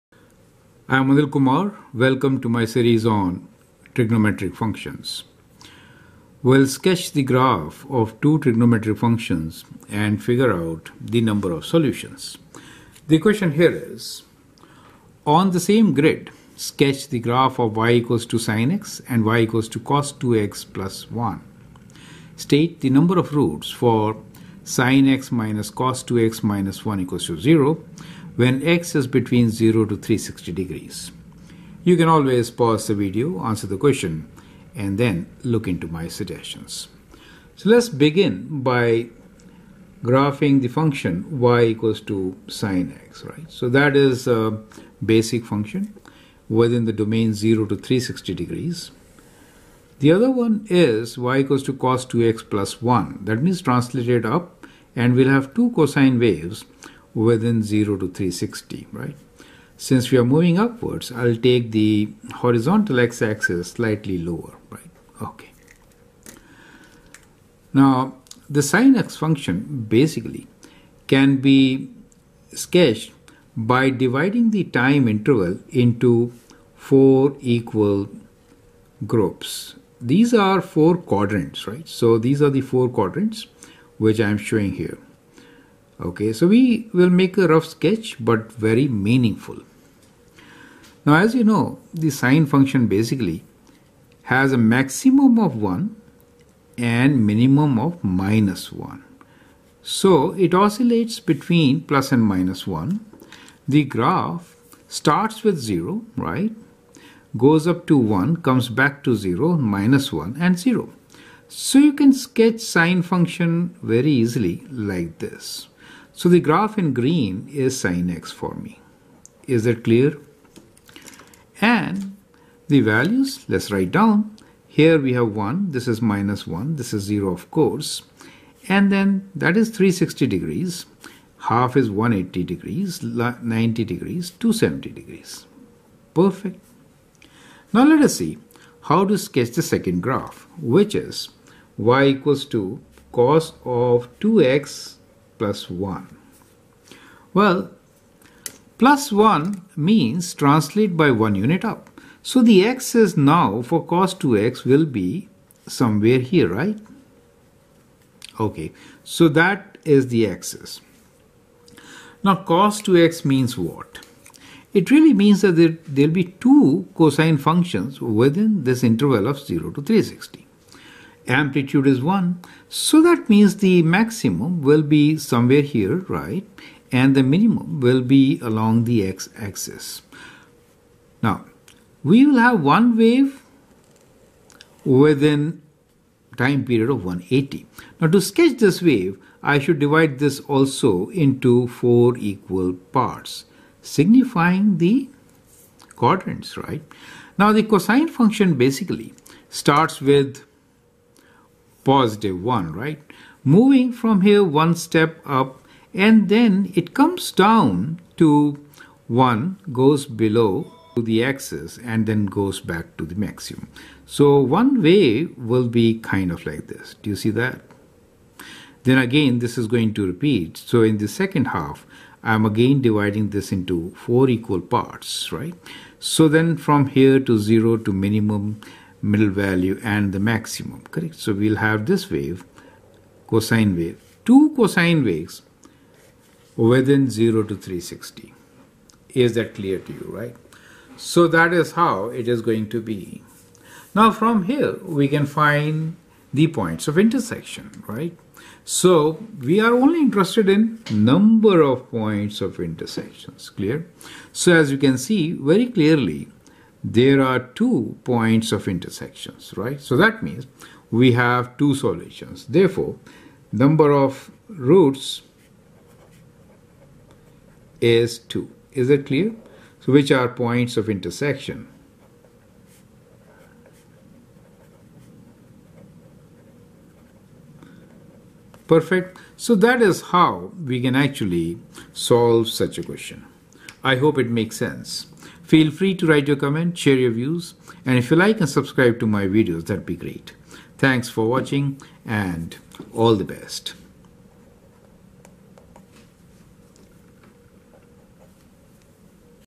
Graph and Find Number of Roots sinx-cos2x-1=0,0°≤x≤360° Trigonometric Equation
Challenge Examples on Compound Angles